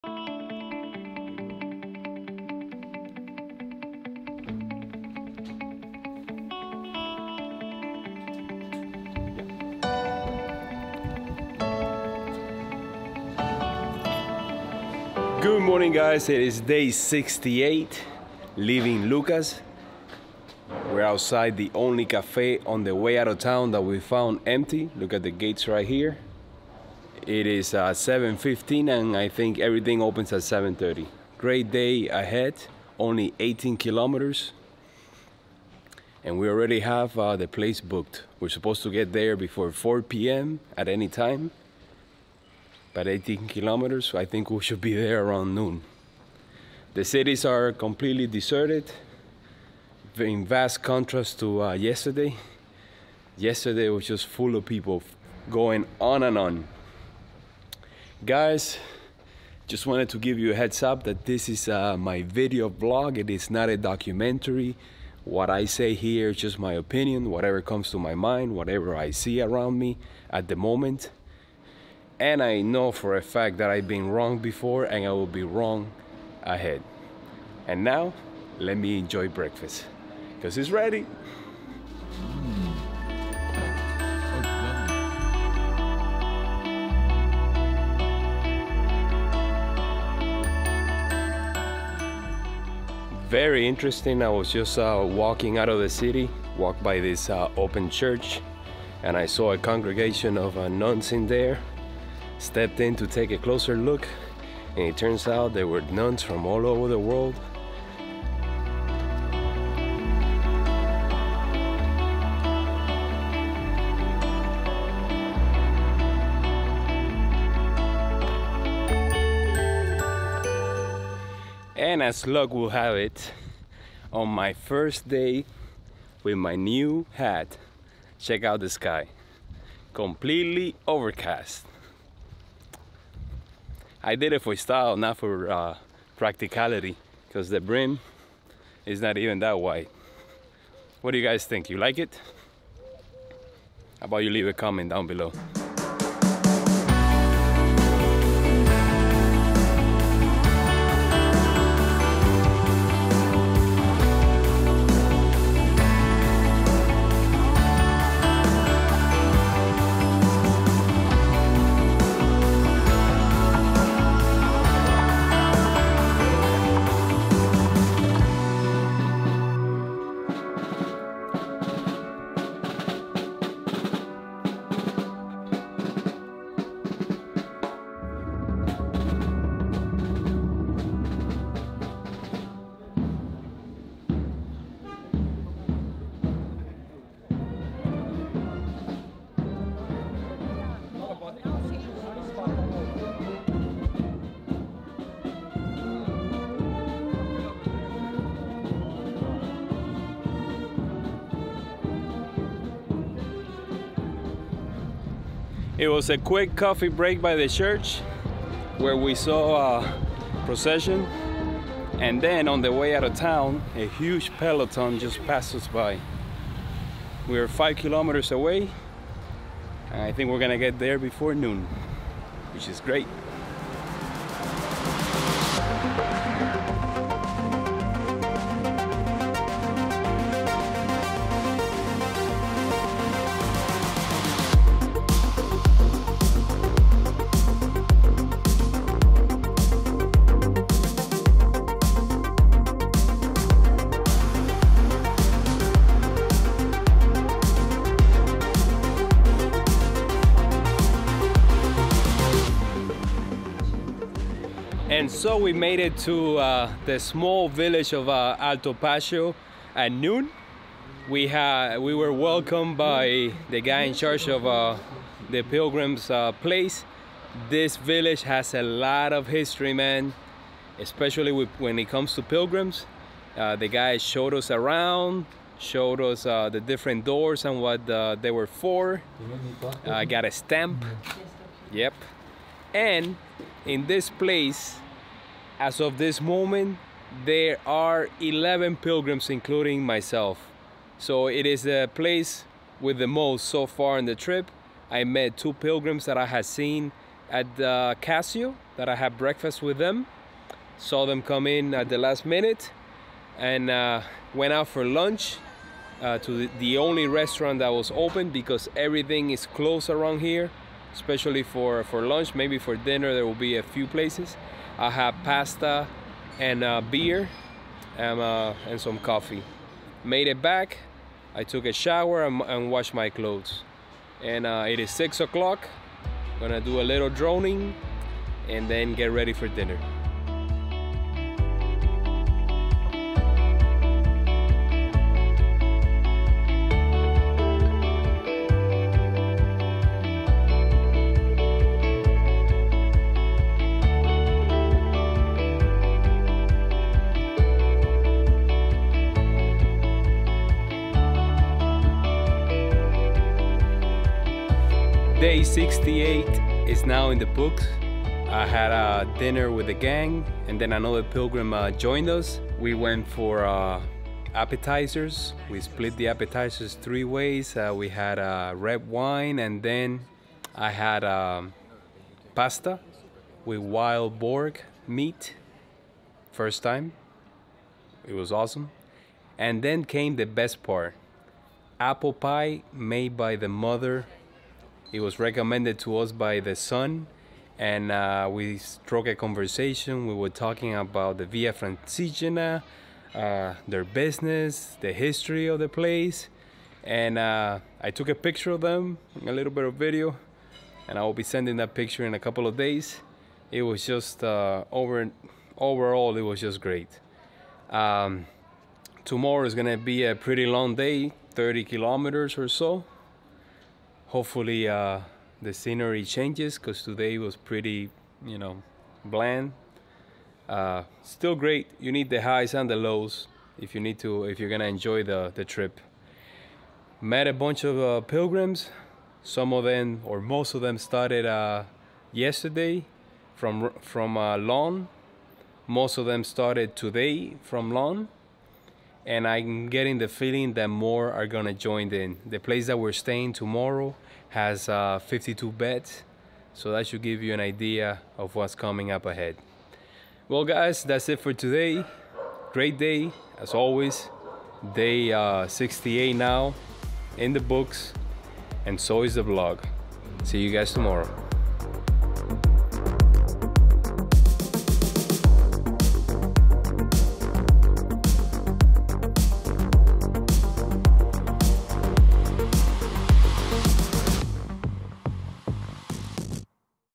68 | Via Francigena in Italy | Lucca to Altopascio, 18.1 km
Daily Cost Breakdown
€05.90 Breakfast
€16.00 Lunch
€10.00 Ostello
€31.90 Total

Accommodation: Magione dei Cavalieri del Tau